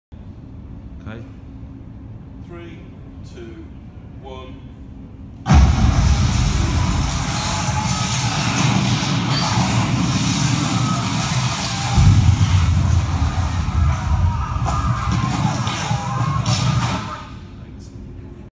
Downton Abbey: Blowing Up
Downton Abbey Travelling Exhibition: In this video, Dan is mixing the sound in the Library, which is a three-sided immersive projection environment with multi-channel audio. Video content by Centre Screen